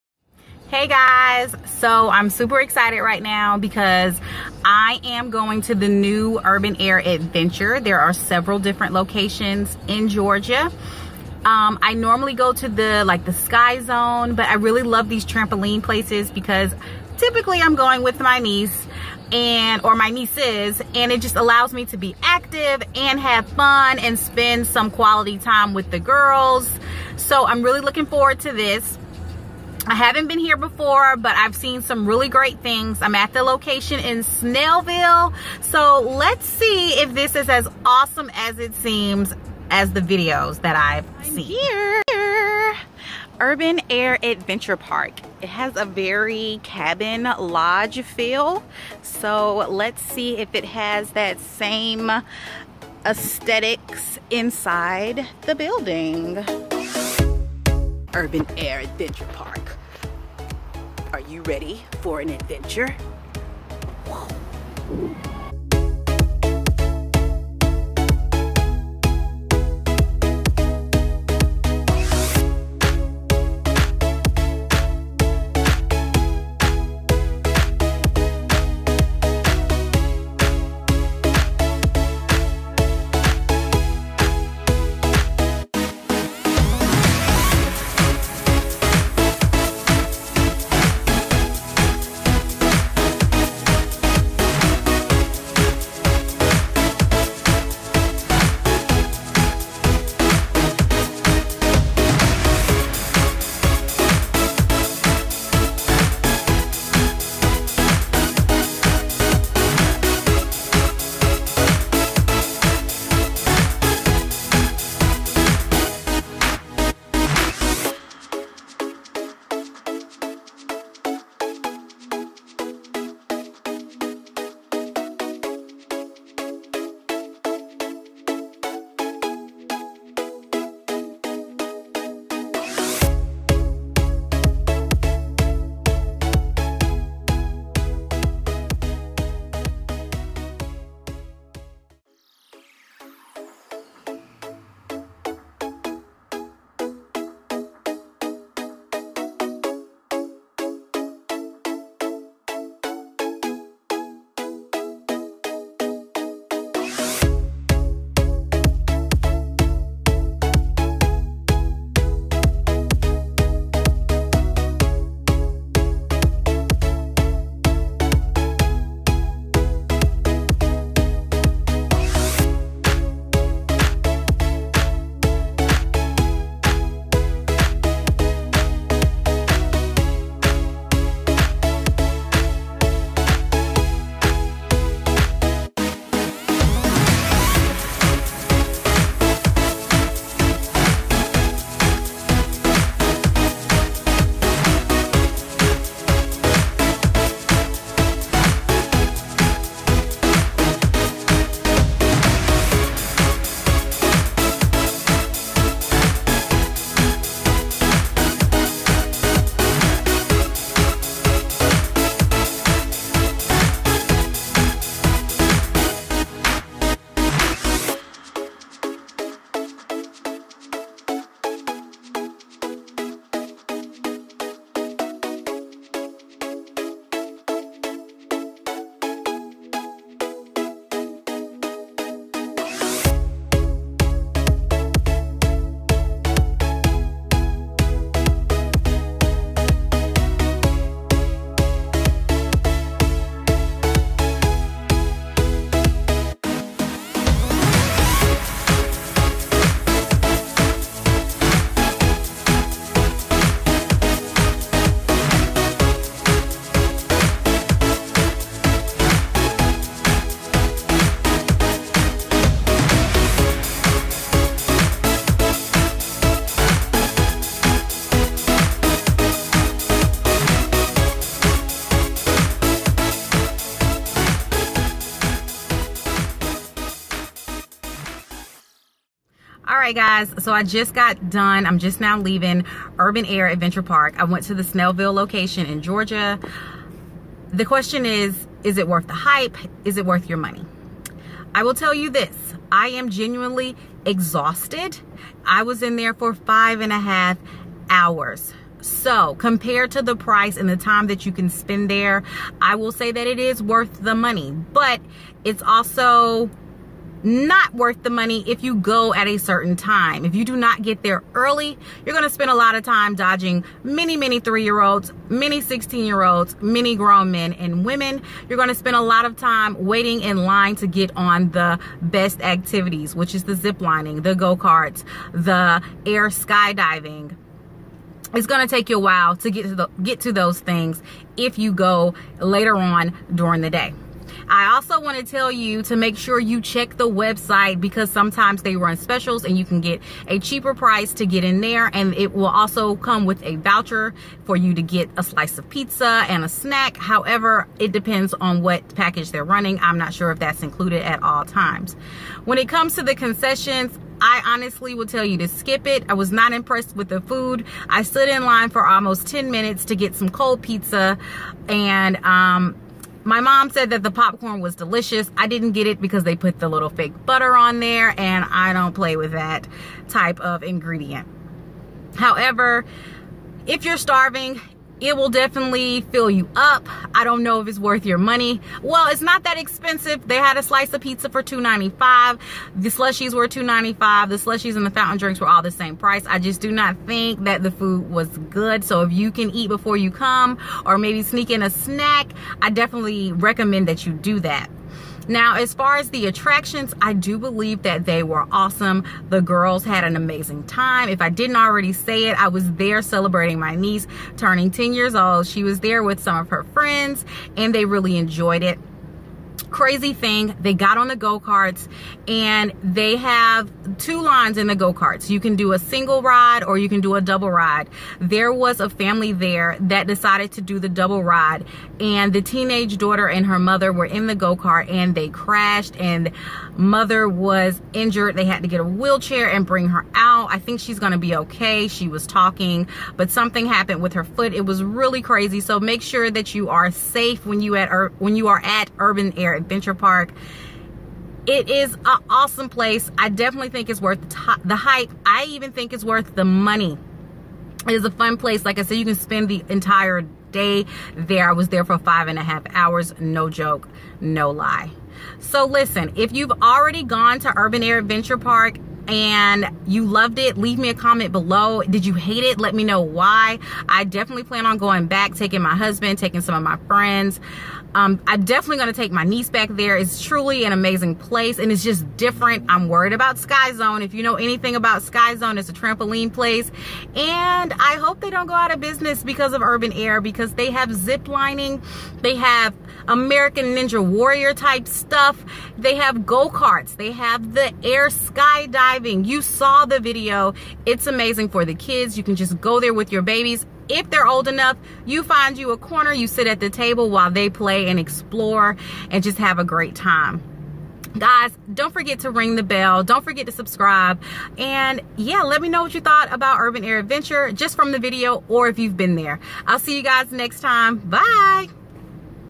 A look inside Urban Air Adventure Park | Is it worth your money?
So I went to Urban Air Adventure Park, but was it worth the money. Watch the video to find out.